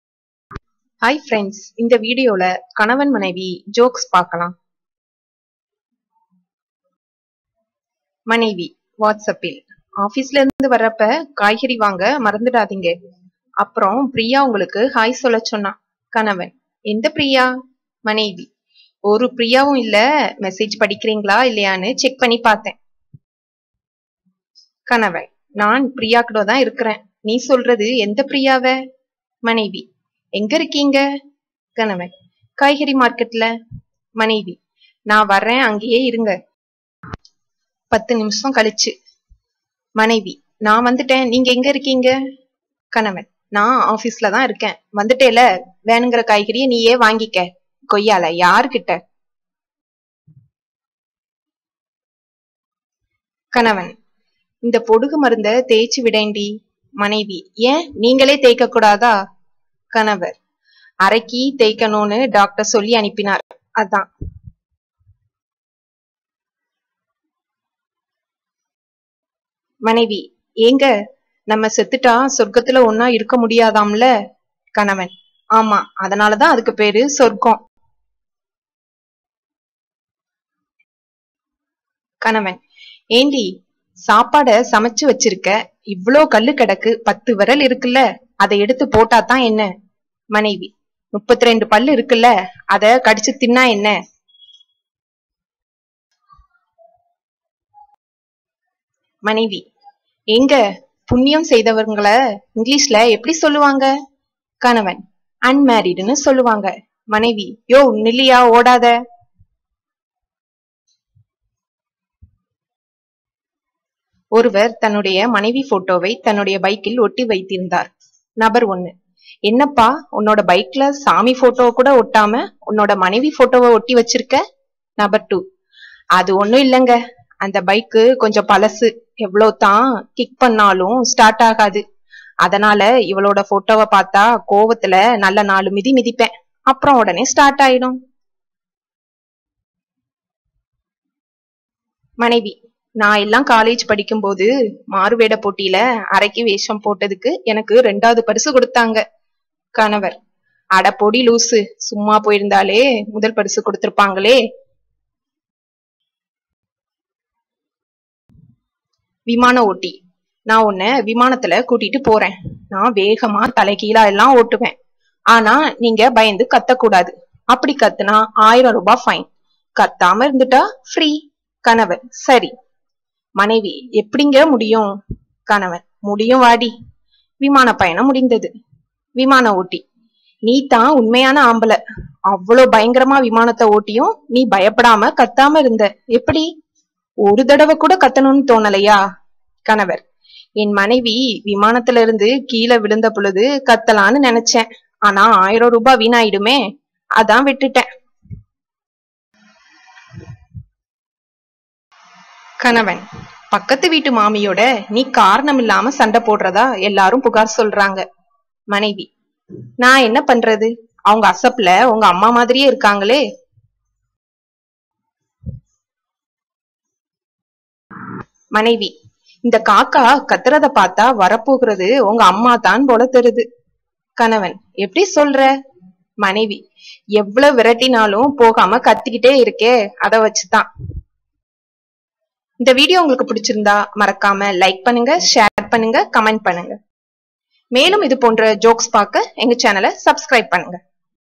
Husband & Wife Jokes : Tamil
Husband & Wife Jokes in Tamil | கணவன் மனைவி ஜோக்ஸ்
Kanavan, Manaivi Jokes

கணவர் : இந்த பொடுகு மருந்தை தேய்ச்சி விடேன்டி!
மனைவி :  ஏன்,  நீங்களே தேய்க்க கூடாதா?
கணவர் : "அரக்கி" தேய்க்கணும்னு டாக்டர் சொல்லி அனுப்பினார், அதான்.